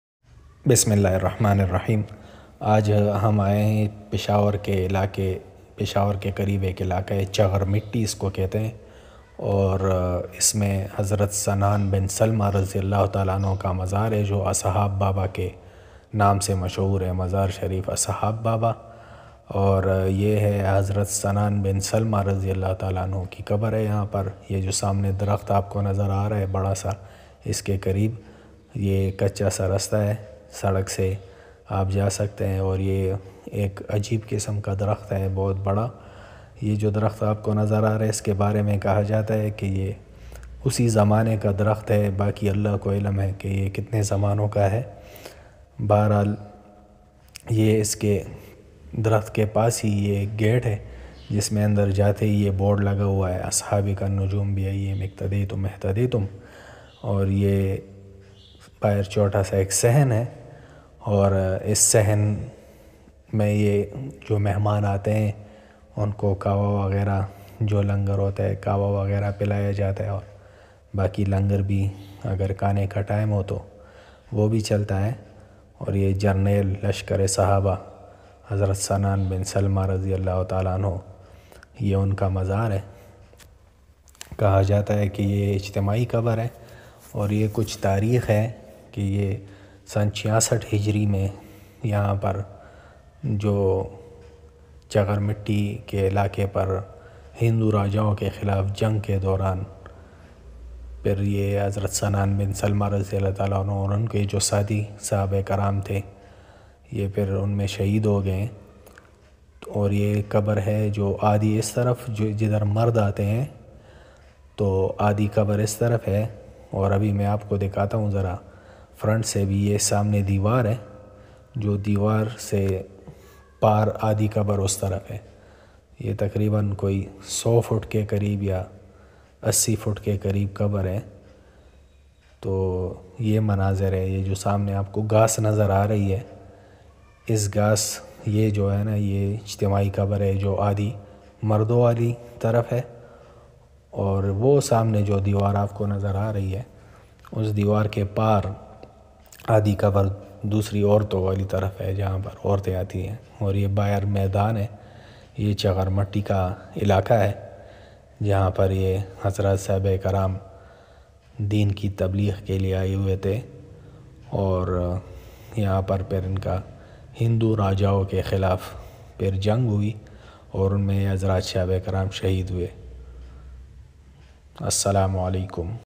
Ashab Baba Peshawar Historical Place | Hazrat Sanan Bin Salma Ka Mazaar | Sahaba Ki Qabar
This Is Very Rare  And Historical Area of Peshawar chaghar Matti Ashab Baba Grave of Hazrat Sanan Bin Salma Sahabi E Rasool.